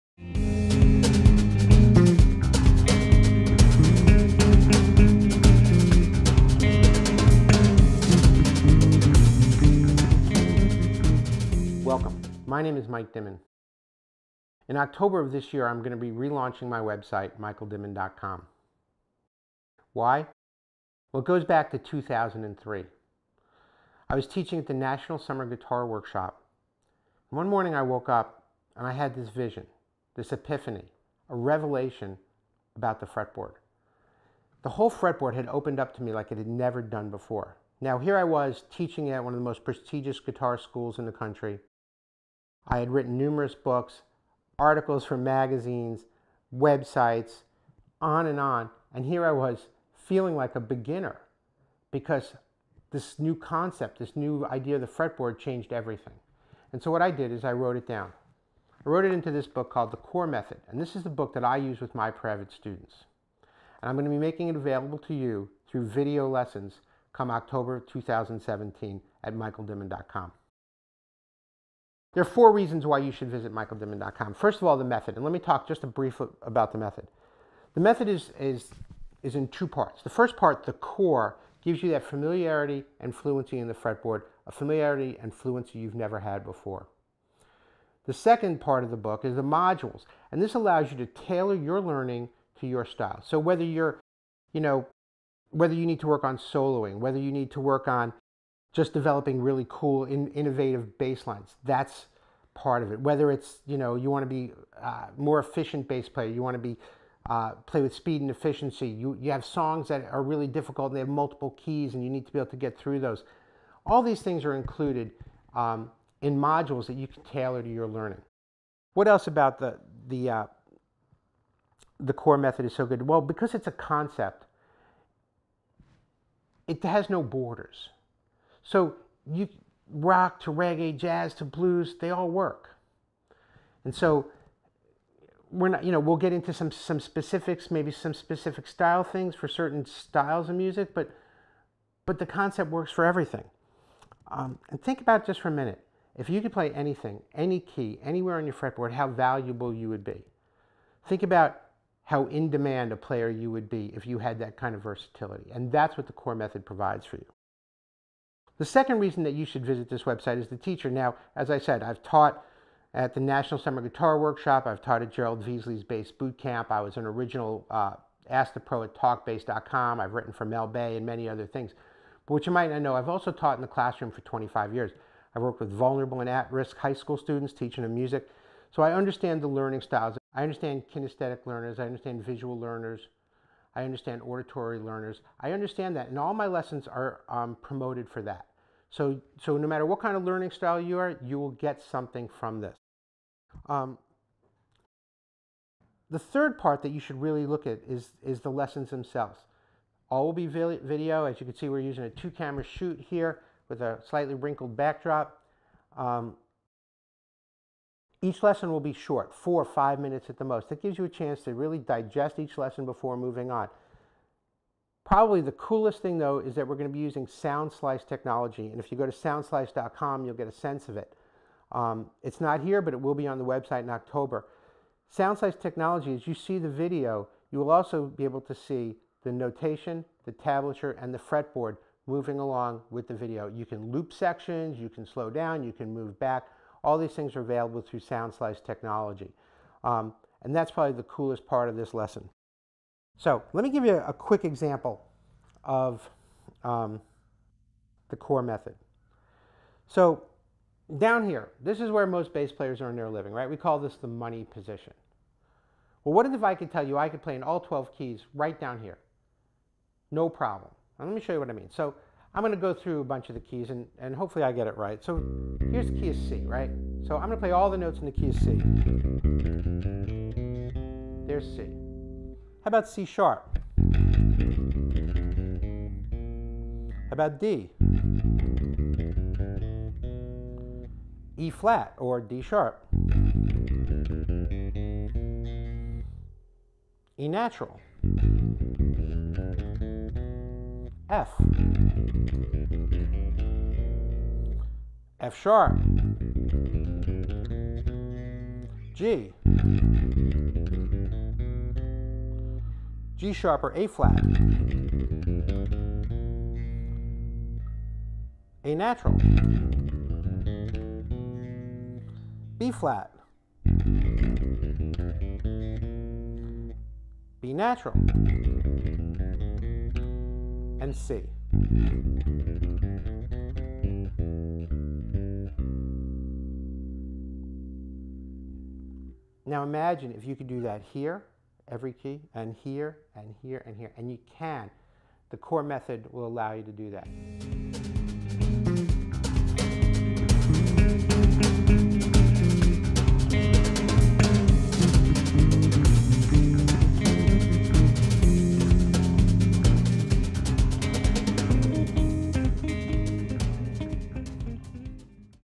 Michael Dimin's The CORE Method
The most innovative, complete and comprehensive bass lesson site on the web by acclaimed author, educator and bassist Michael Dimin! If you have struggled with finding Bass Lessons online that fit your schedule or your learning needs that is all about to change.  Michael Dimin has released his groundbreaking new lessons based on The CORE Method. This video is a quick intro from Mike that explains the who, what, when, where and why of the CORE Method and the Soundslice technology.